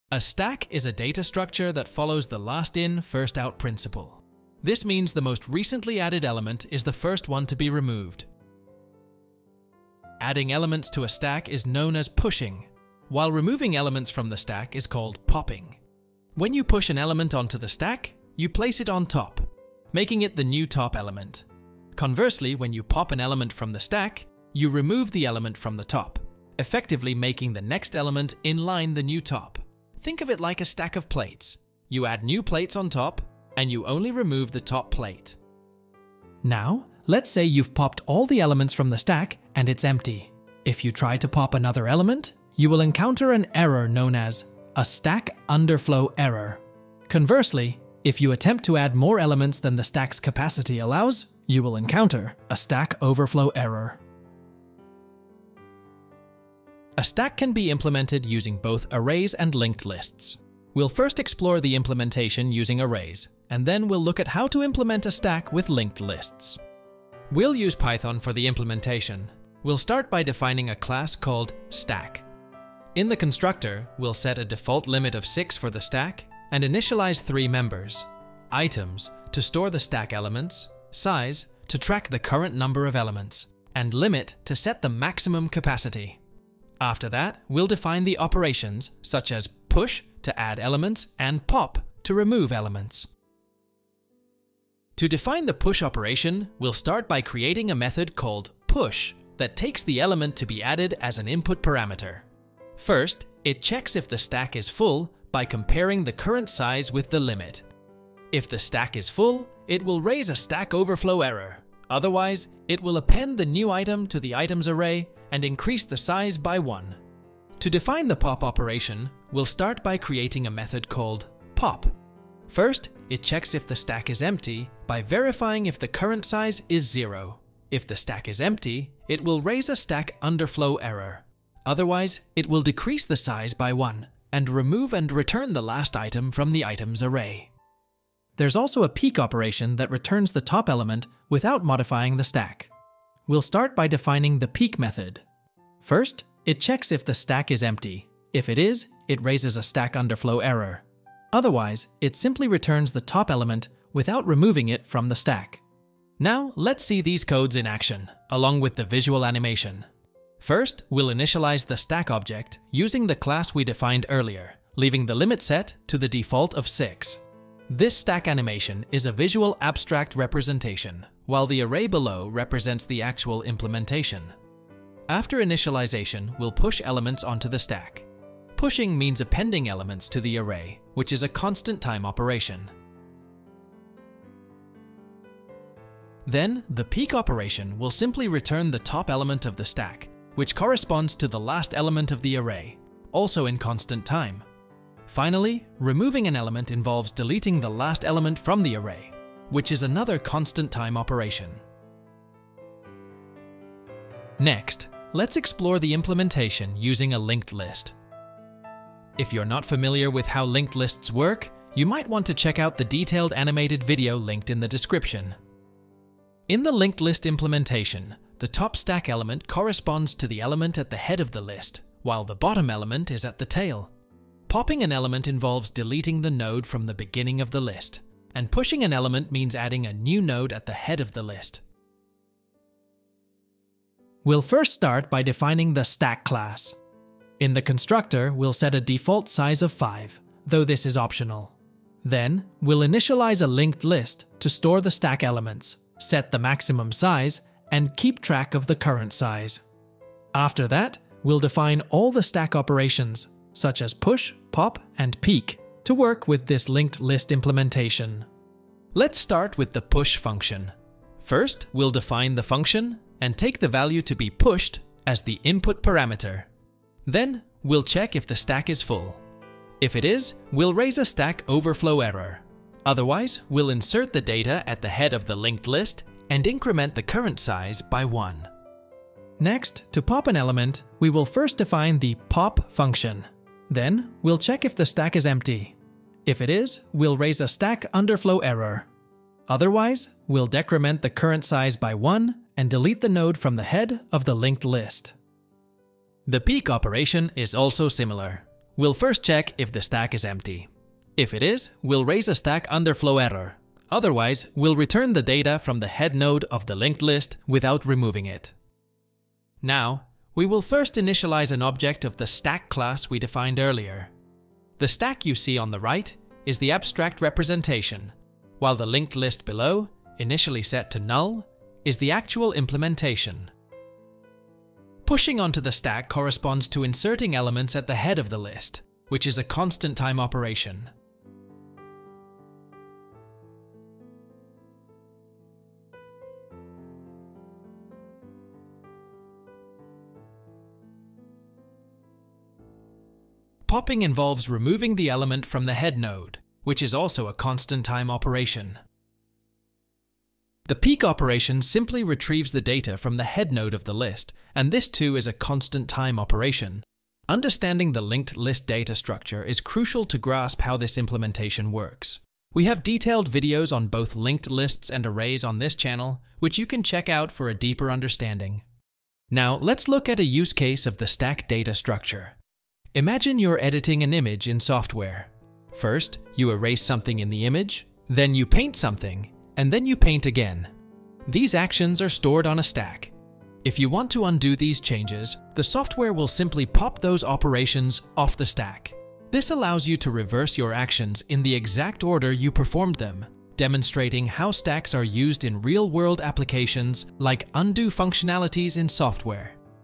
Stack Visually Explained
this video contains a visual explanation of the stack data structure. the video wastes no time and only focuses on quick demonstrations and explanations.

First, the basics of the stack data structure have been explained, and then its actual implementation with both arrays and linked lists.

understanding the implementations will require a proper understanding of Arrays and Linked Lists. the "Visually Explained" videos of those two are:-